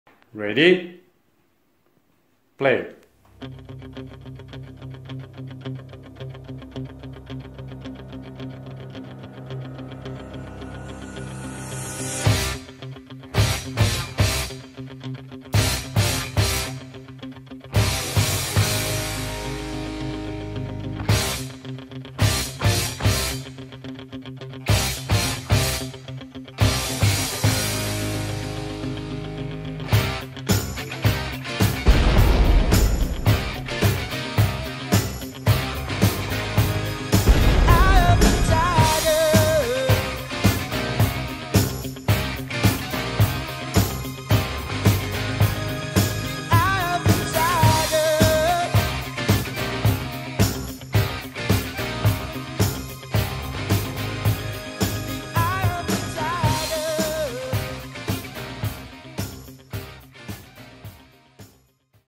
Nole 311 world record
Tribute video, our way to celebrate Novak Djokovic 311 weeks at no 1 !!!